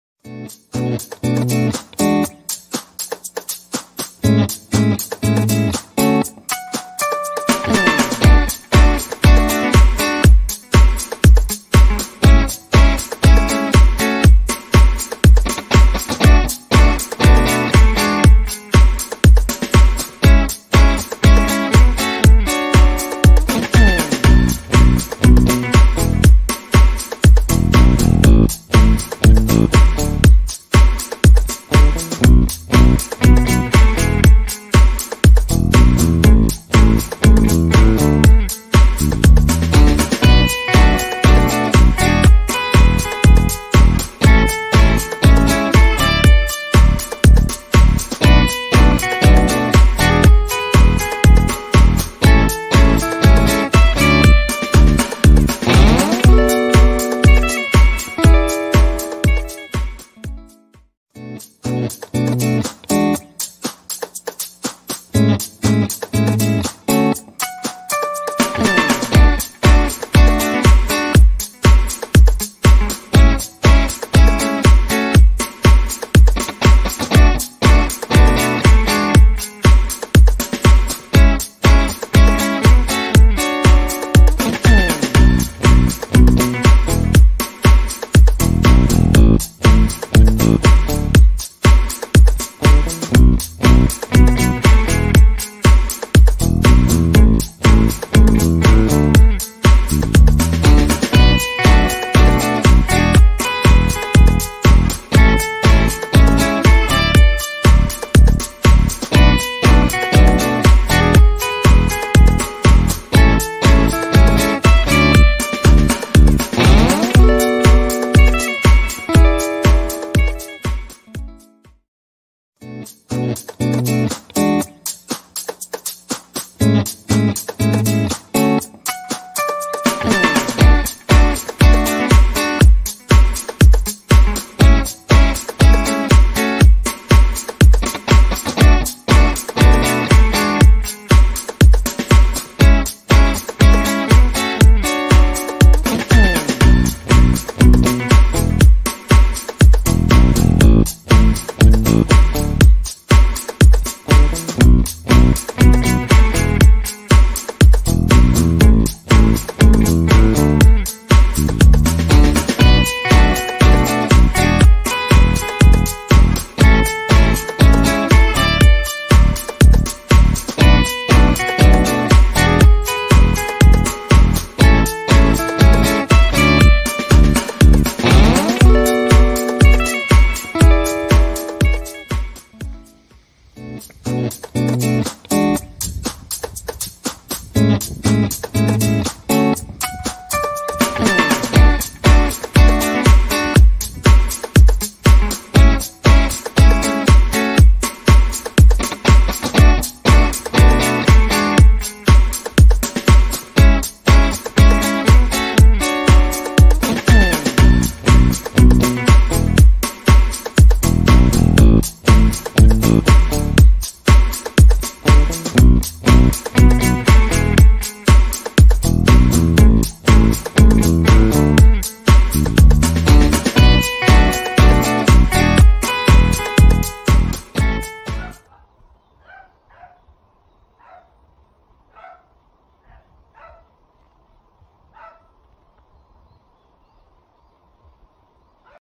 廃材を利用してパーゴラを作ってみました🎵
既存の鉄パイプを利用してパーゴラを作りました🎵ローコスト目指して庭作りを楽しんでいます。
制作費用
メッシュワイヤー
415円×3枚
錆びの上から塗れる塗料
1000円弱